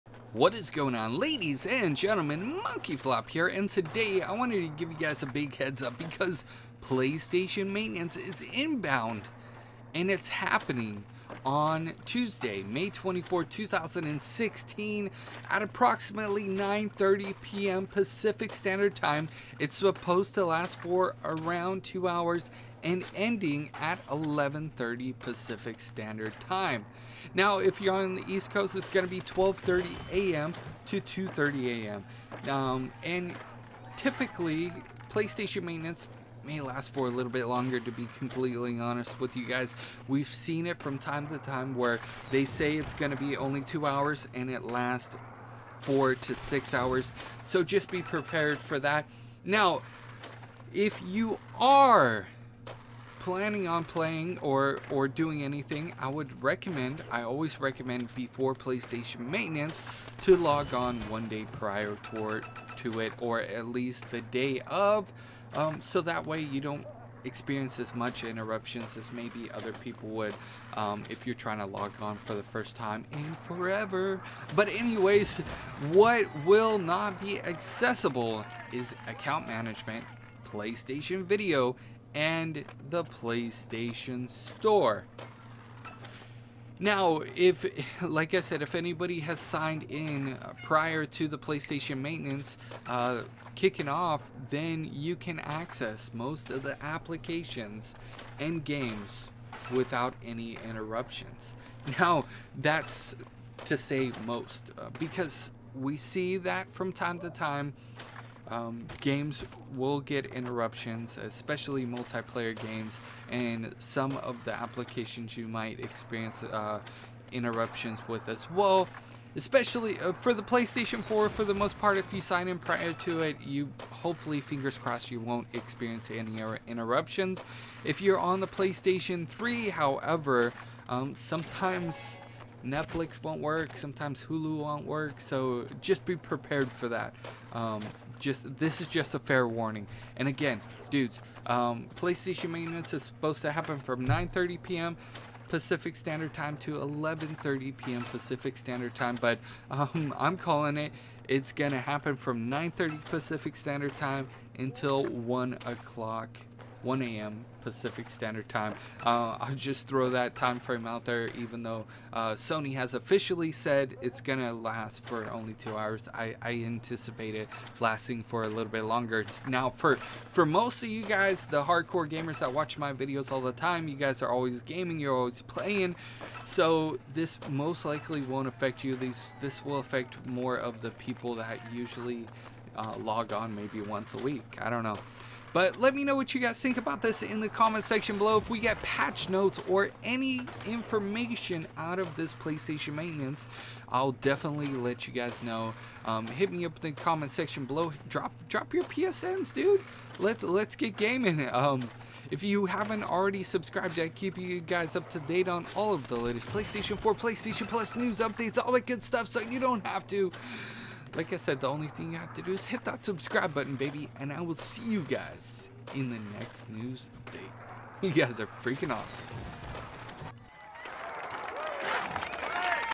PSN Maintenance Scheduled May 2016 Cant login to PS4 Account Managment & More
PSN PS4 PS3 PS Vita Maintenance Schedule May 2016 Cant log on to PS4 Account Management PS Video & MorePlayStation News Daily with MonkeyFlop

Game On!!
Family Friendly